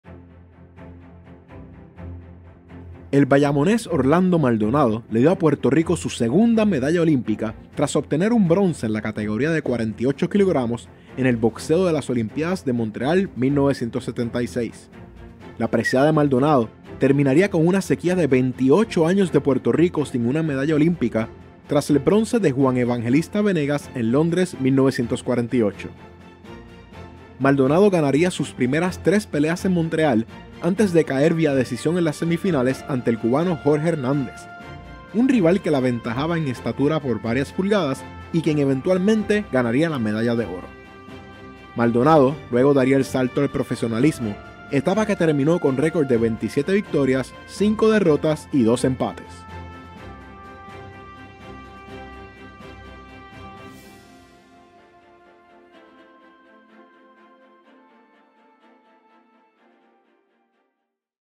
El día en que Orlando Maldonado ganó la segunda medalla olímpica para Puerto Rico
Así el boxeador boricua se alzó con bronce en las Olimpiadas de Montreal 1976.